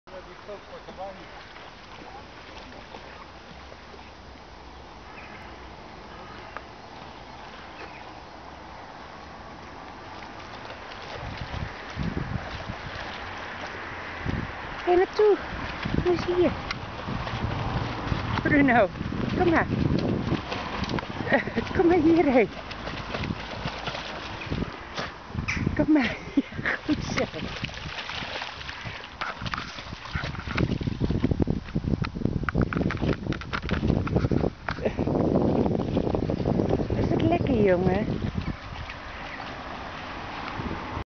BRUNO (Podenco) WANTS TO SWIM
Podenco's are not that kind of types, who like to swim. But seeing birds. Bruno goes!!